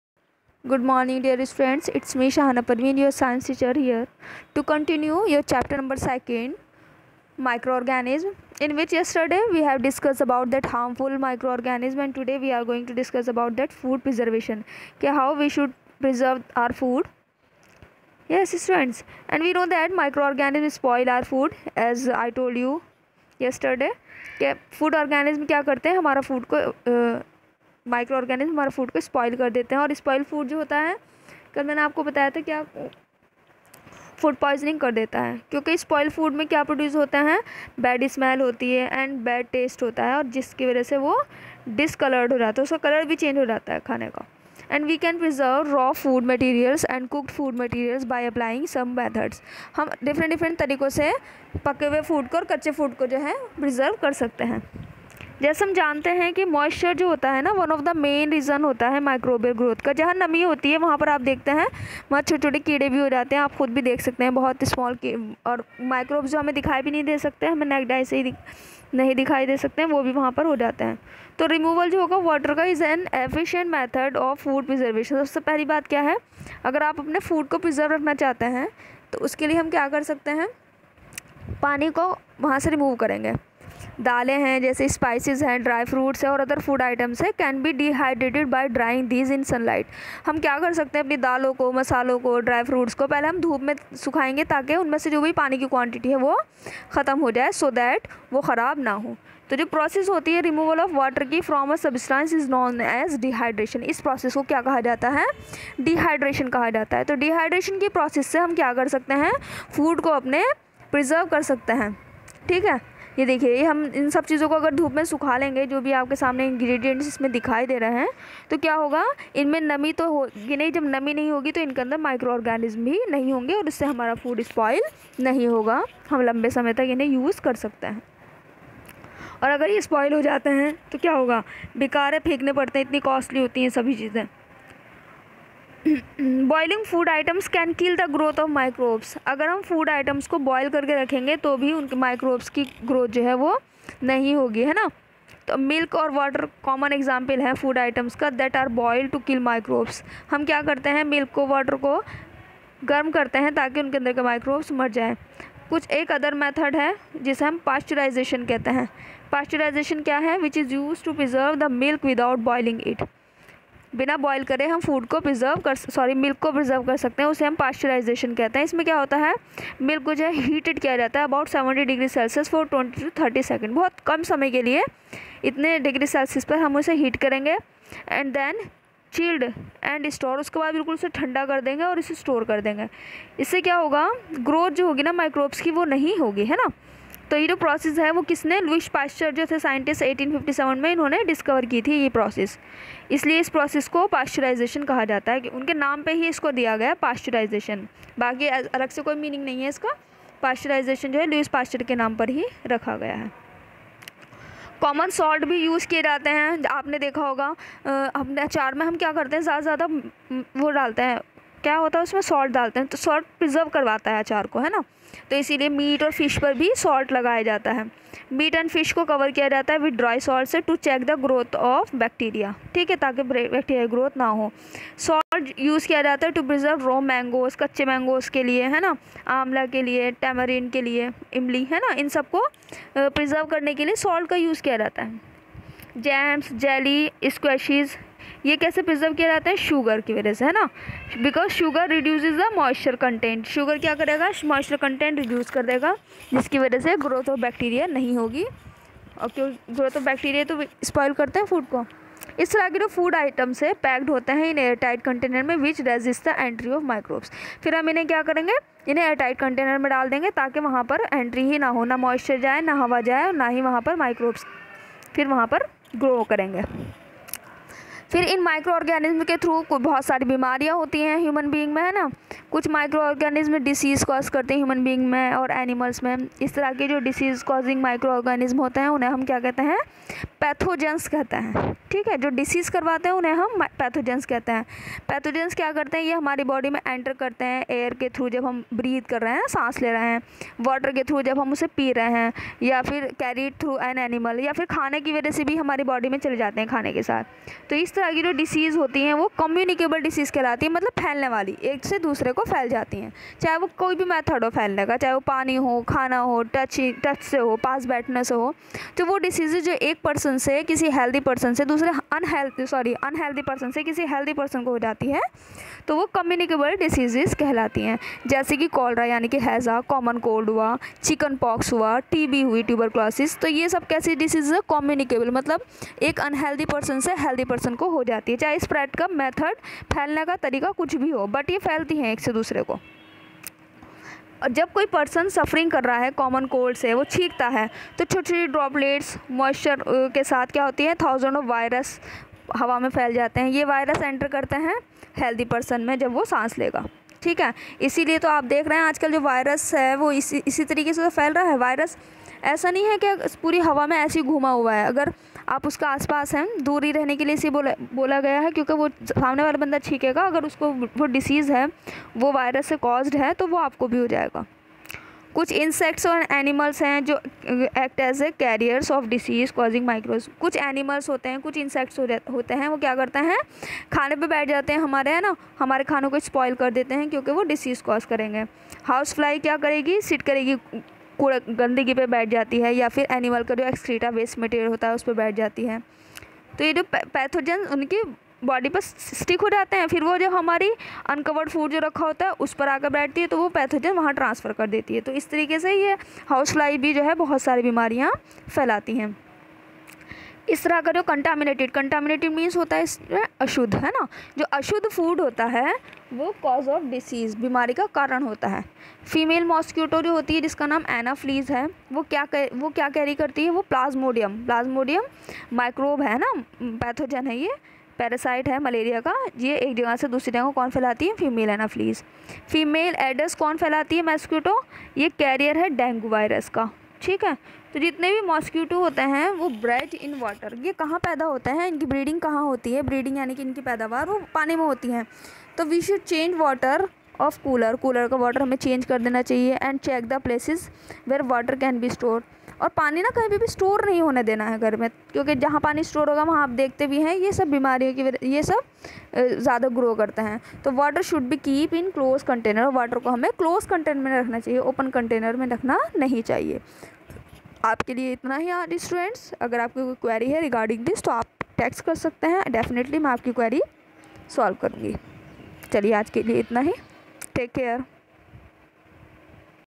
Class-8th.. ( Chapter-2 C ) Micro organisms
Food preservation and diseases caused by micro organisms